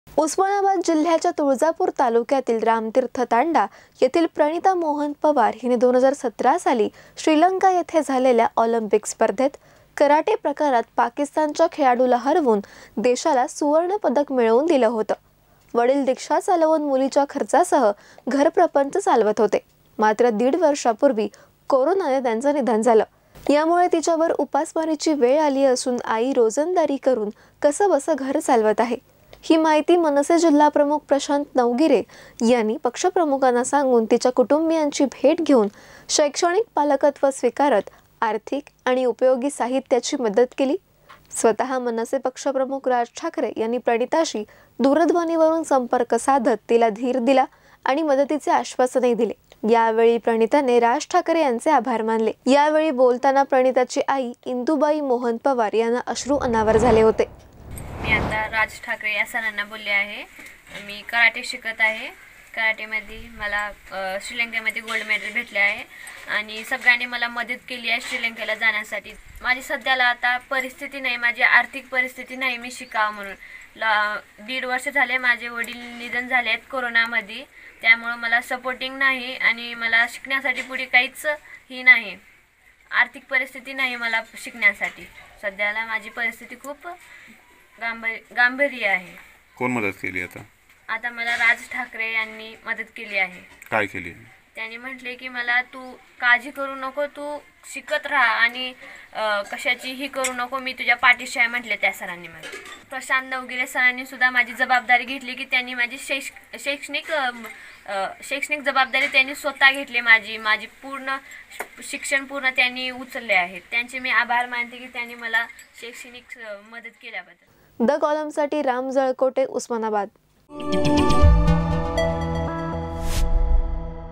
Osmanabad | २०१७ च्या ऑलिम्पिक स्पर्धेत भारताला सुवर्णपदक मिळवून देणाऱ्या खेळाडूवर उपासमारीची वेळ
उस्मानाबाद जिल्ह्याच्या तुळजापुर तालुक्यातील रामतीर्थ तांडा येथील प्रणिता मोहन पवार हीने २०१७ साली श्रीलंका येथे झालेल्या ऑलिम्पिक स्पर्धेत कराटे प्रकारात पाकिस्तानच्या खेळाडूला हरवुन देशाला सुवर्णपदक मिळवून दिले होते...वडील रिक्षा चालवून मुलीच्या खर्चासह घरप्रपंच चालवत होते...मात्र दीड वर्षापुर्वी कोरोनाने त्यांचं निधन झालं...यामुळे तिच्यावर उपसमारीची वेळ आली असून आई रोजंदारी करून कसबस घर चालवत आहे...ही माहिती मनसे जिल्हाप्रमुख प्रशांत नवगिरे यांनी पक्षप्रमुखांना सांगून तिच्या कुटूंबीयांची भेट घेऊन शैक्षणिक पालकत्व स्विकारत आर्थिक आणि उपयोगी साहित्याची मदत केली...स्वतः मनसे पक्षप्रमुख राज ठाकरे यांनी प्रणिताशी दुरध्वनीवरून संपर्क साधत तिला धीर दिला आणि मदतीचे आश्वासन दिले...यावेळी प्राणिताने राज ठाकरे यांचे आभार मानले...यावेळी बोलताना प्रणिताची आई इंदुबाई मोहन पवार यांना अश्रू अनावर झाले होते...